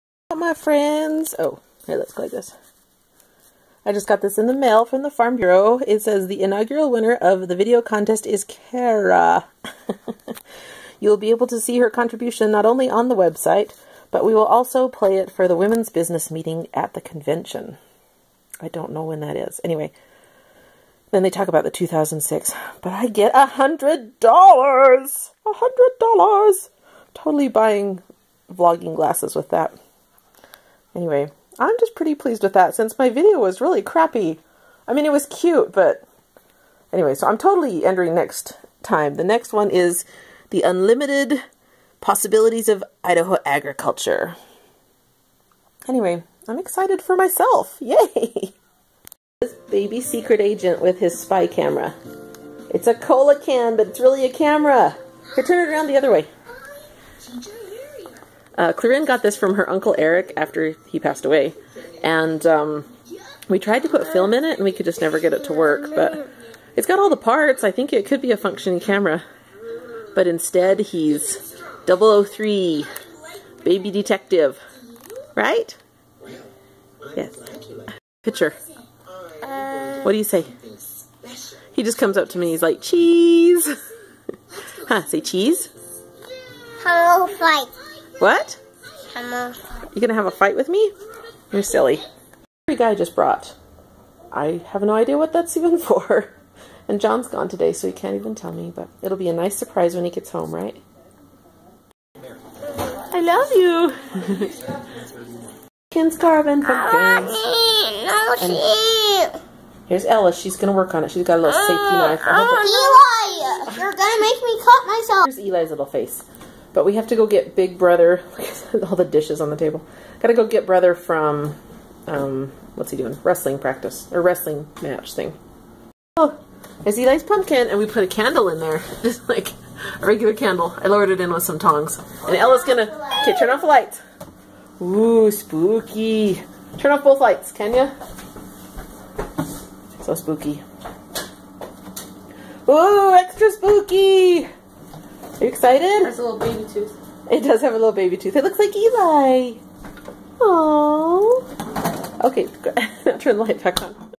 I WON A VIDEO CONTEST! 805
No biggie, but I won $100!  yay!  Vlogging sunglasses, here I come!  I guess a super agent COULD go look for the video on the Farm Bureau of Idaho web site, IF it's there.  I haven't actually looked for it.